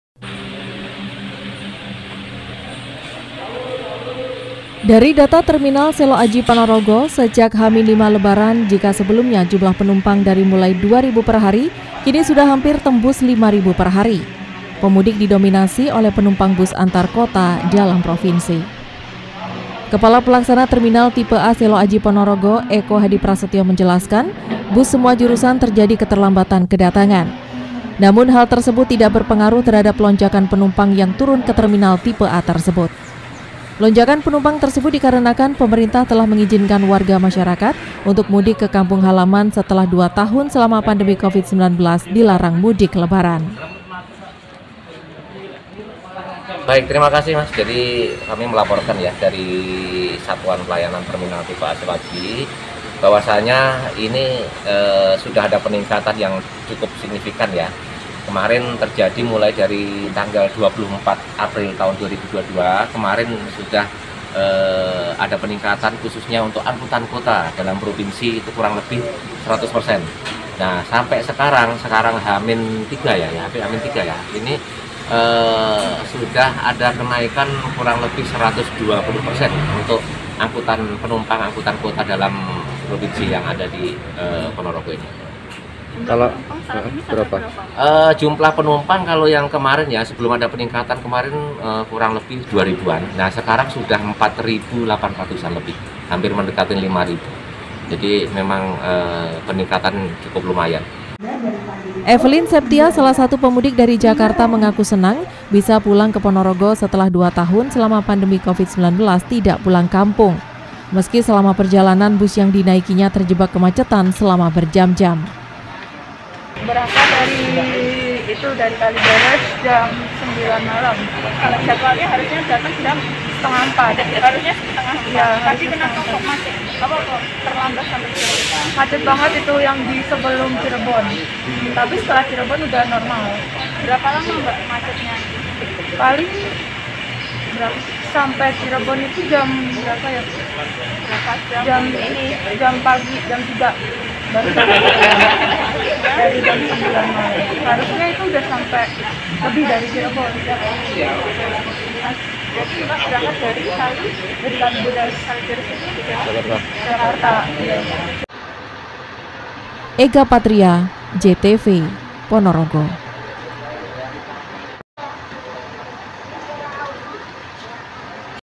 PONOROGO - Pemudik Mulai Padati Terminal Seloaji, Kenaikan Dua Kali Lipat
Ribuan pemudik mulai memadati terminal seloaji ponorogo. Bahkan lonjakan penumpang terjadi sejak h-5 lebaran dengan jumlah dua kali lipat.